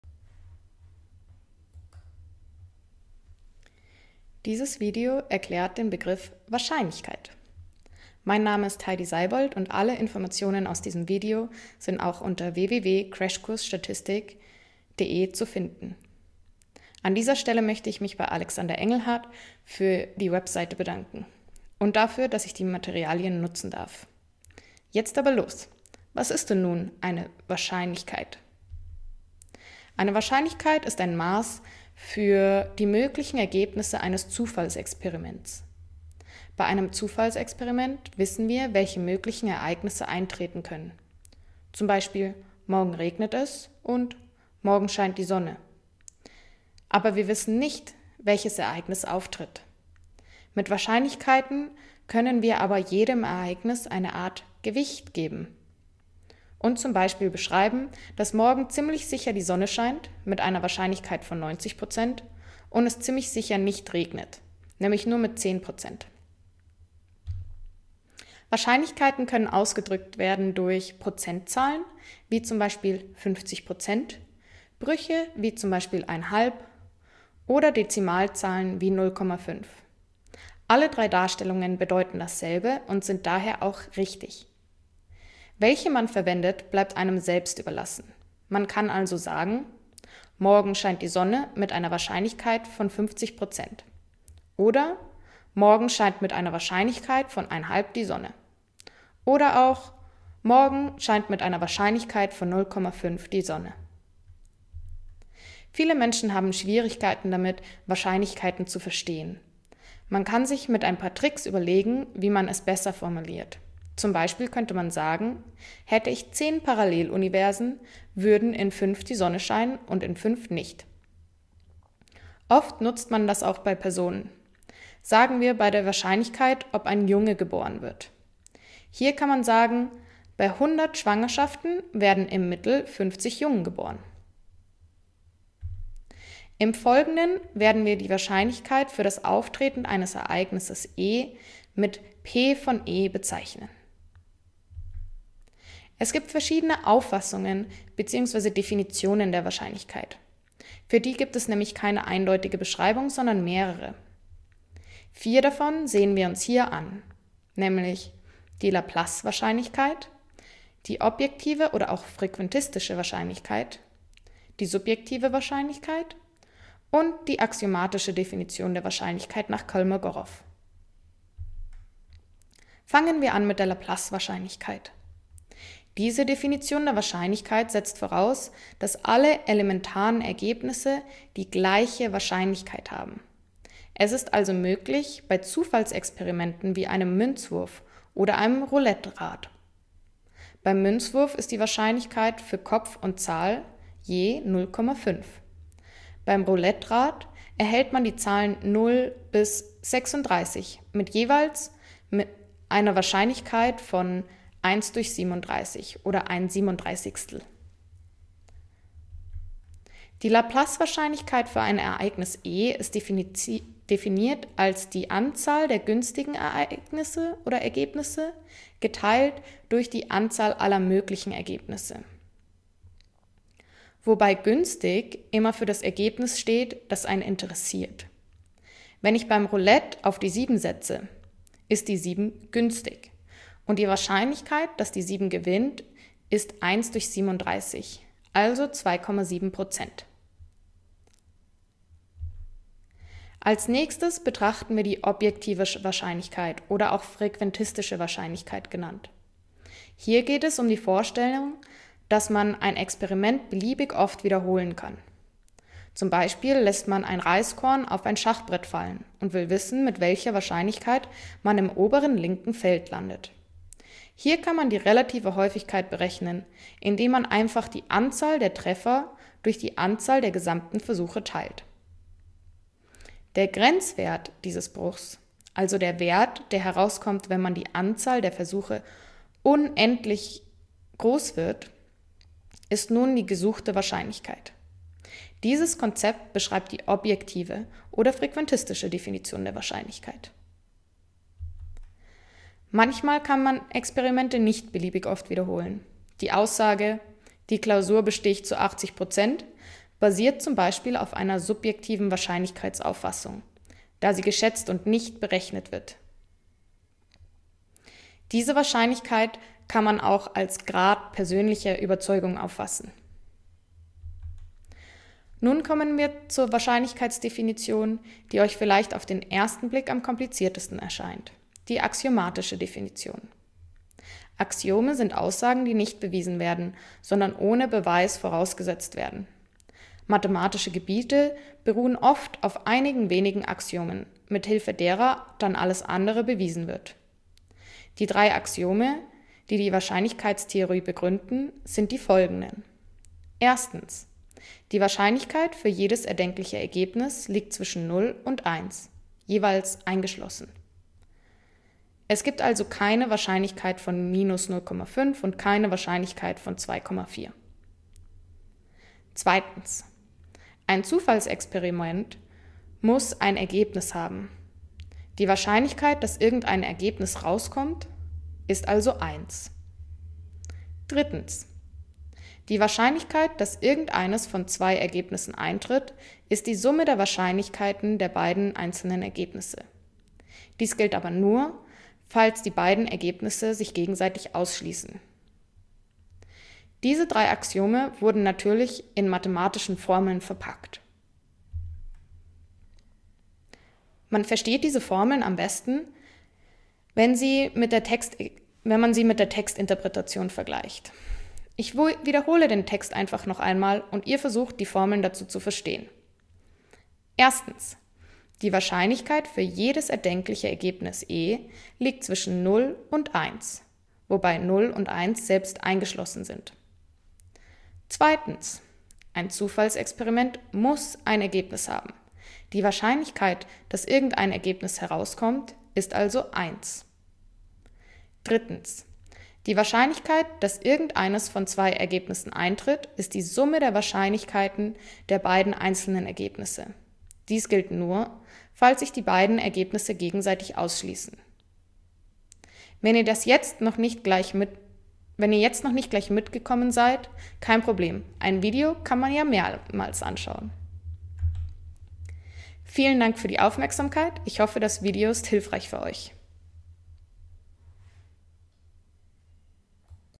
Was ist eine Wahrscheinlichkeit?
Definitionen der Wahrscheinlichkeit:

- Laplacewahrscheinlichkeit
- Objektive oder frequentistische Wahrscheinlichkeit
- Subjektive Wahrscheinlichkeit
- Axiomatische Definition (nach Kolmogorov)